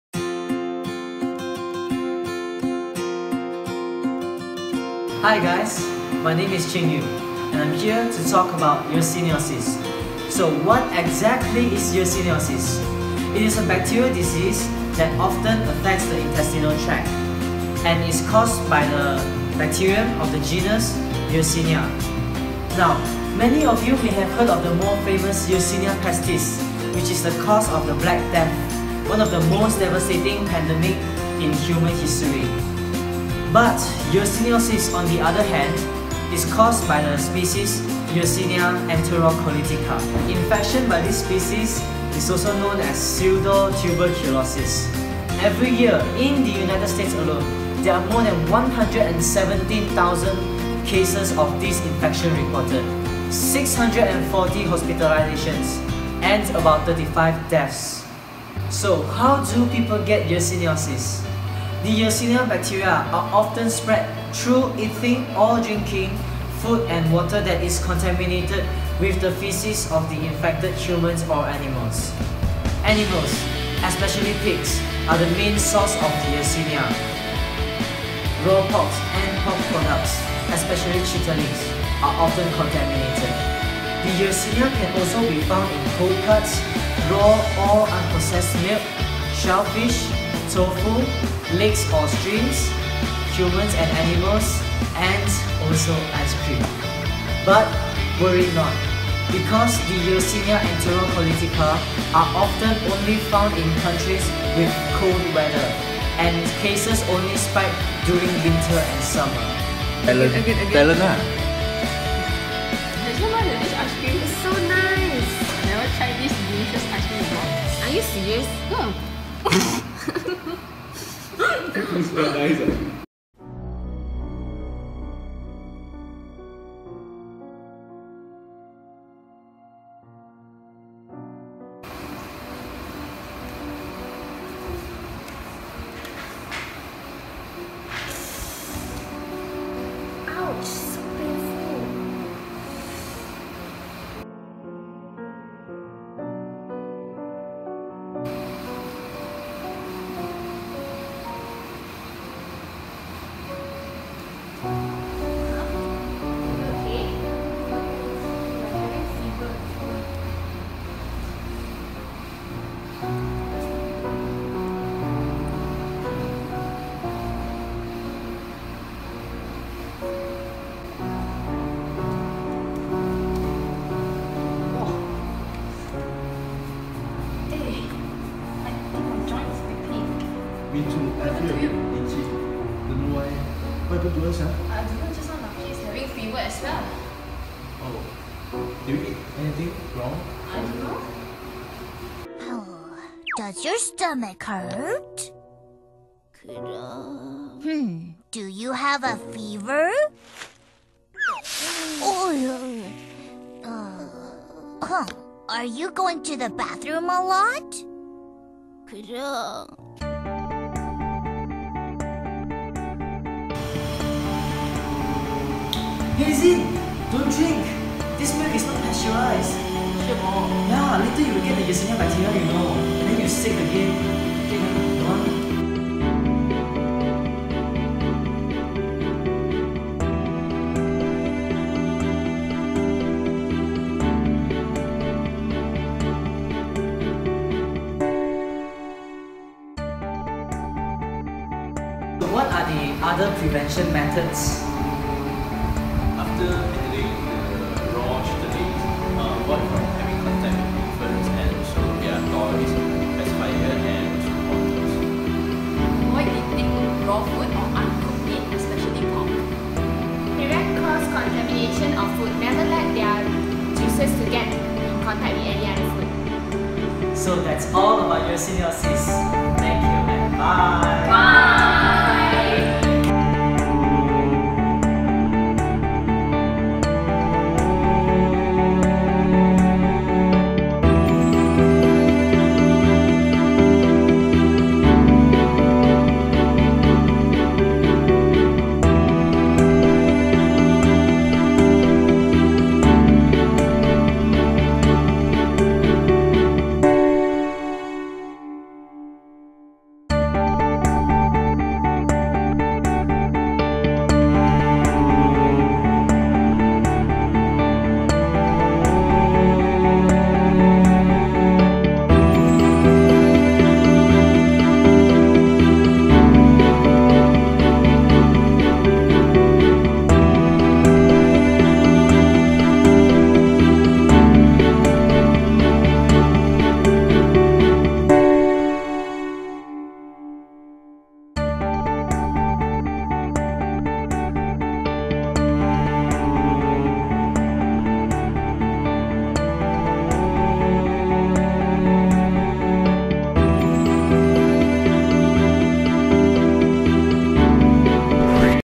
Yersinia Enterocolitica (microbiology)
A short video about the bacteria yersinia enterocolitica of the bacterium genus Yersinia, and the infection that it causes, the yersiniosis.